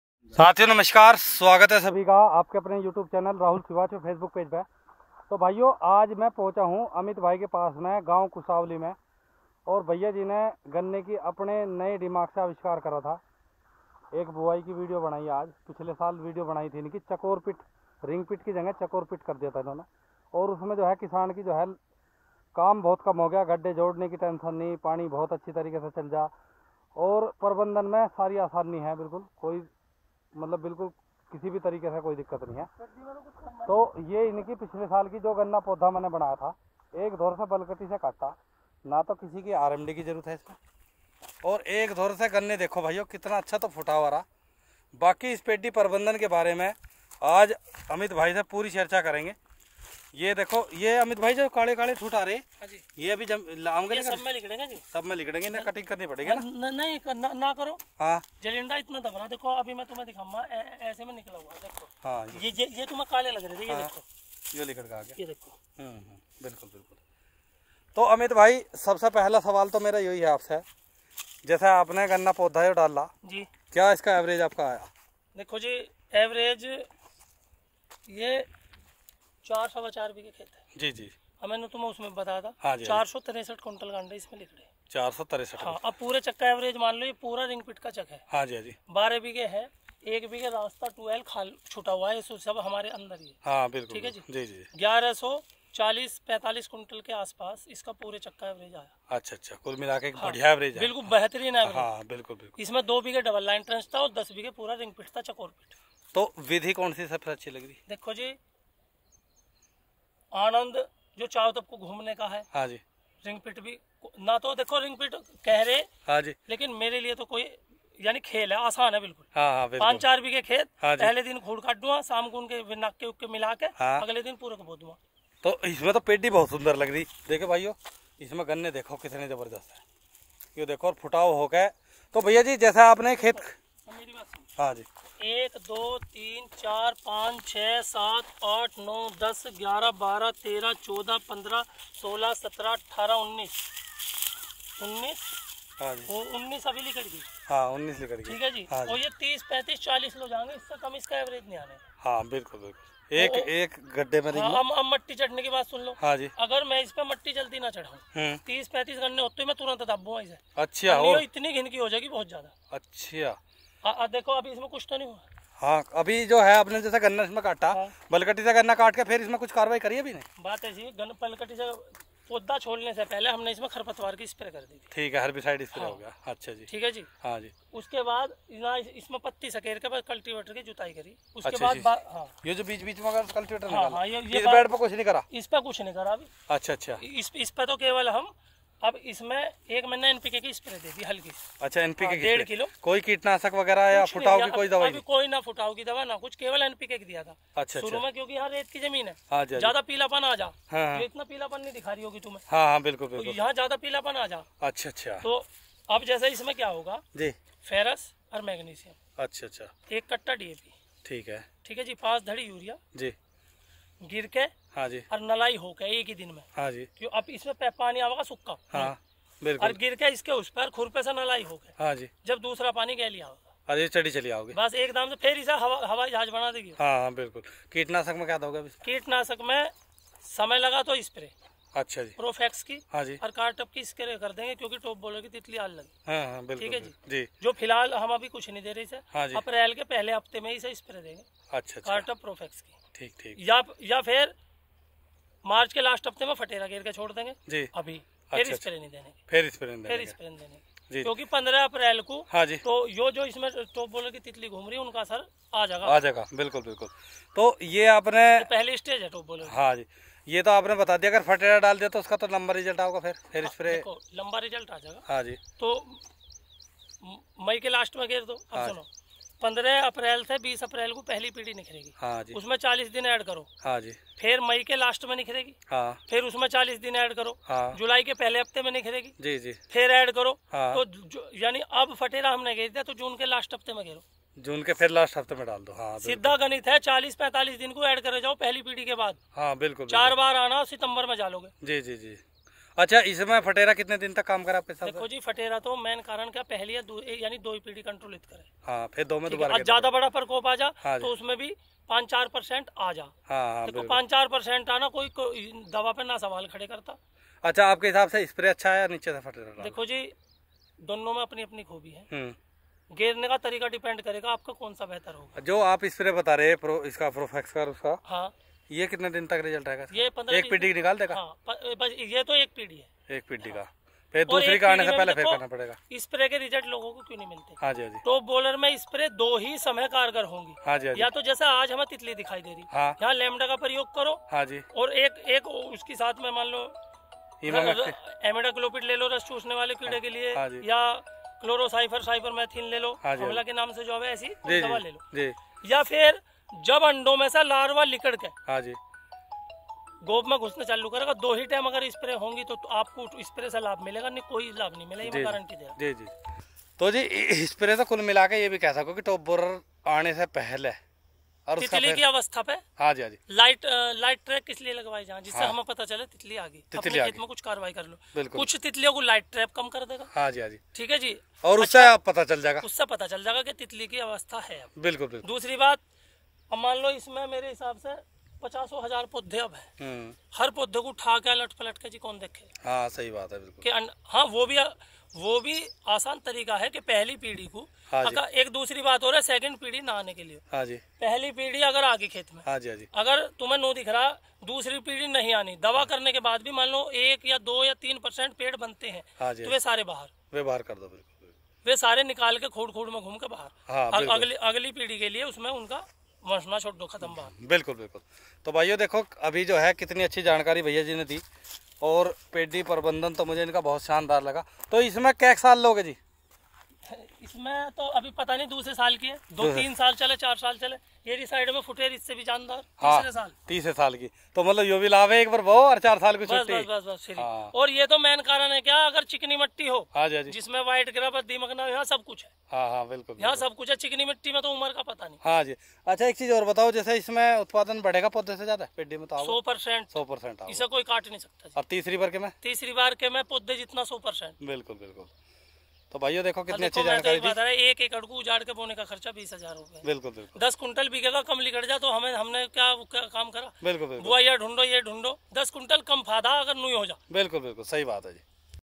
देखिए अमित भाई का चकोर पीट विधि द्वारा गन्ना पेड़ी 4 से 5 साल तक ले पेड़ी 100 प्लस प्रति बीघा एवरेज
rahulsiwach shortvideo किसान आज ट्रेंच भारतीय गन्ने भाइयों sugarcane देखिए अमित भाई कुशावली वालों की चकोर पीठ की गन्ना पड़ी प्रबंधन इतनी शानदार गन्ना पेड़ी पहले आपने कहीं नहीं देखी होगी